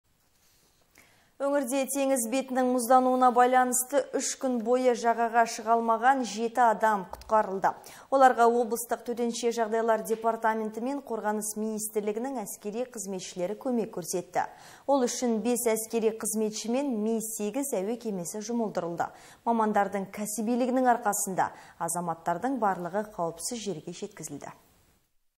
Жағаға шыға алмаған 7 адам құтқарылды
Маңғыстау. Жағаға шыға алмаған 7 адам құтқарылды

30 қараша 2020

Өңірде теңіз бетінің мұздануына байланысты 3 күн бойы жағаға шыға алмаған 7 адам құтқарылды. Оларға облыстық төтенше жағдайлар департаменті мен Қорғаныс министрлігінің әскери қызметшілері көмек көрсетті. Ол үшін 5 әскери қызметші мен Ми-8 әуе кемесі жұмылдырылды. Мамандардың кәсібилігінің арқасында азаматтардың барлығы қауіпсіз жерге жеткізілді.